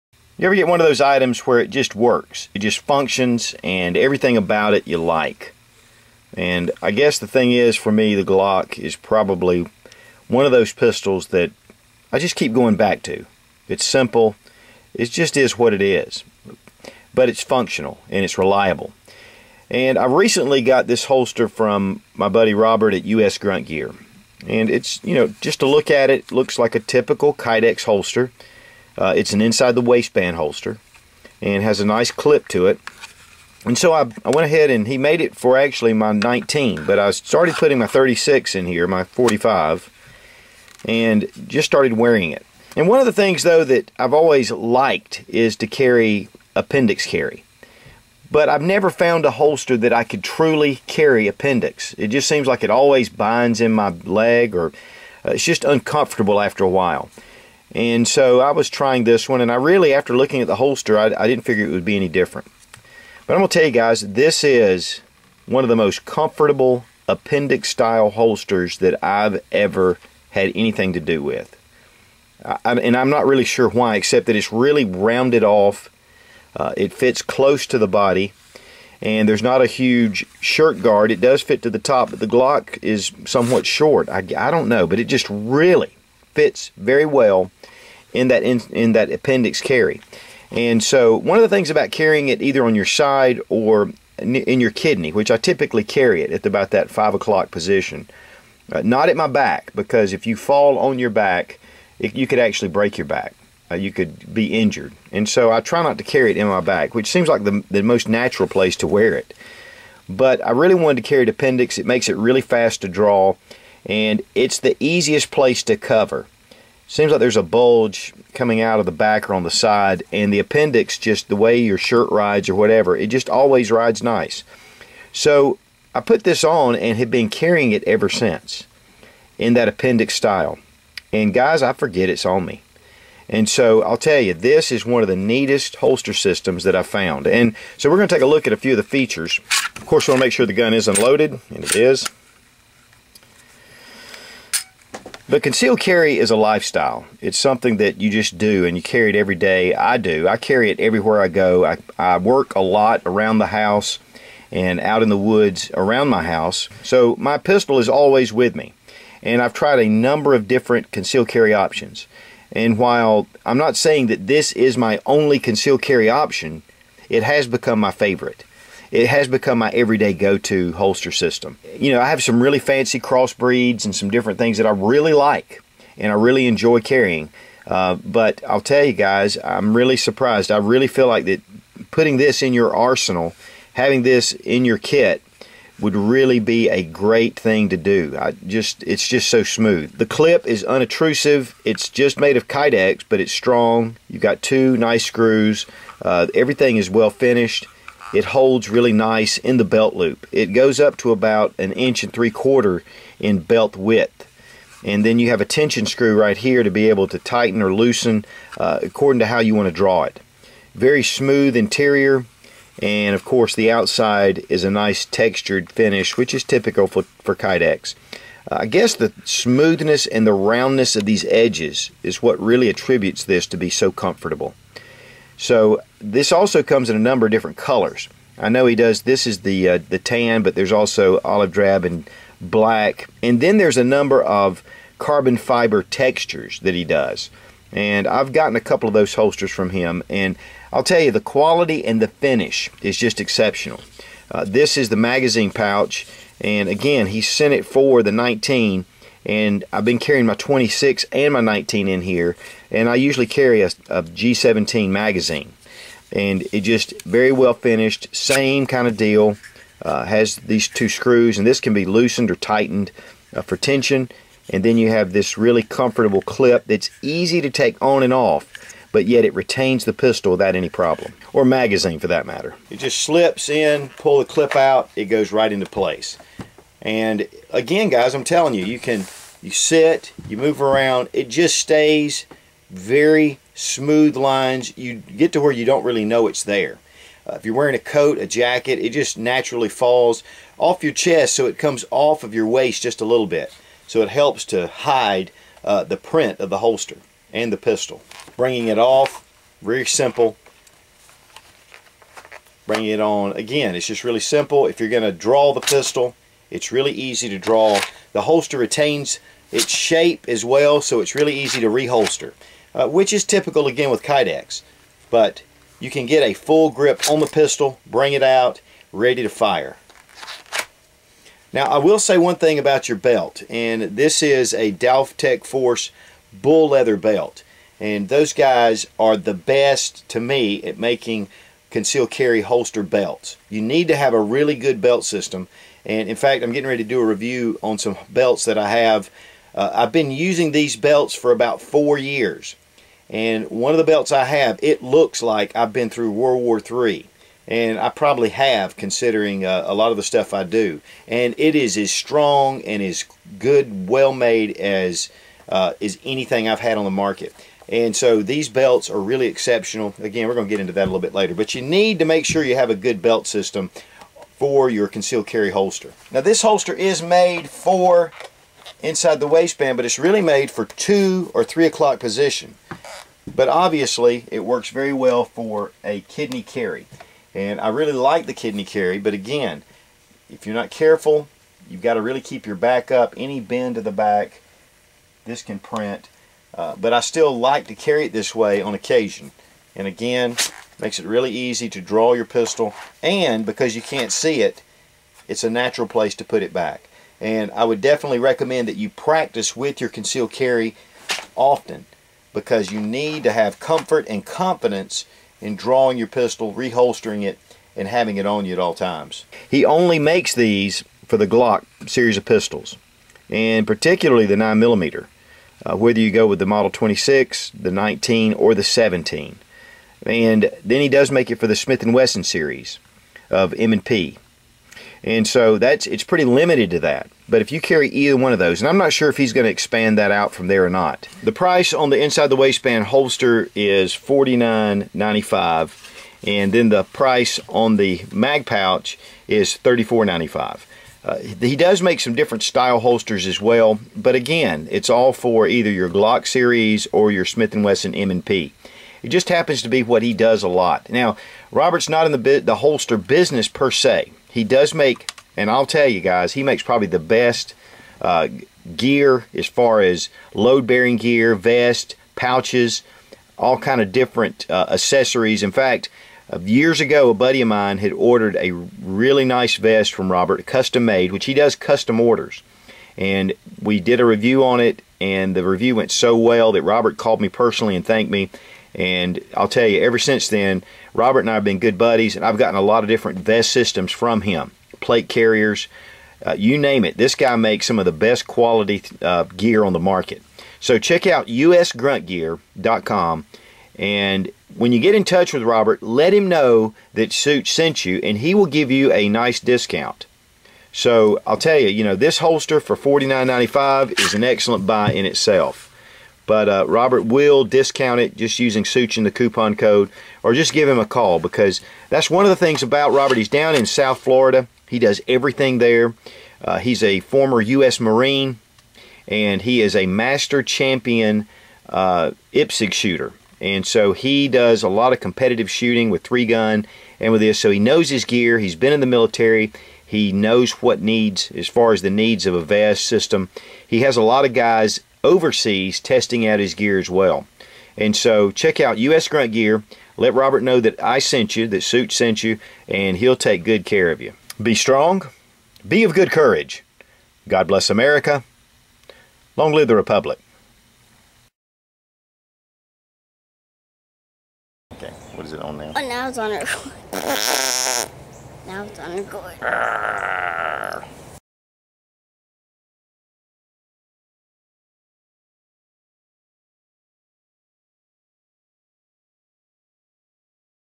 The Best Appendix Carry Holster for the Glock Pistols
Fun Gun Gear Reviews Presents: "The Best Appendix Carry Holster for the Glock 19"  US Grunt Gear makes top quality Tactical Plate Carriers, Chest Rigs and Battle Belts but they also make one of the best IWB Holsters for the Glock and S&W M&P Pistols.

Daltech Force Bull Leather Belts